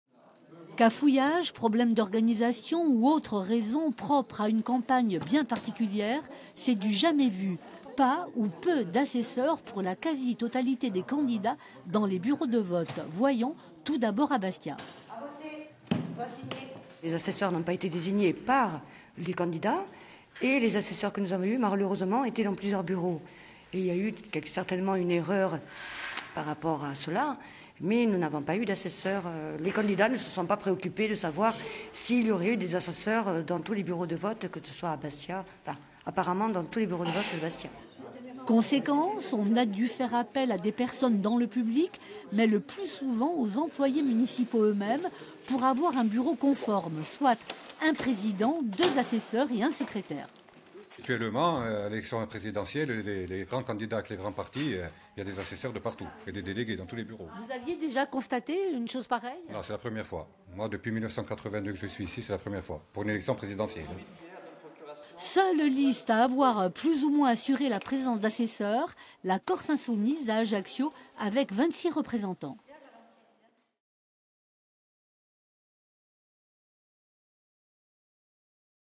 Présidentielle : vote retardé en Corse par manque d'assesseurs
Le bug de ce premier tour de l'élection présidentielle 2017 a eu lieu en Corse, où des bureaux de vote se sont retrouvés sans assesseurs ou en nombre insuffisant dans plusieurs bureaux, retardant d'autant les opérations de vote, jusqu'à démotiver des électeurs.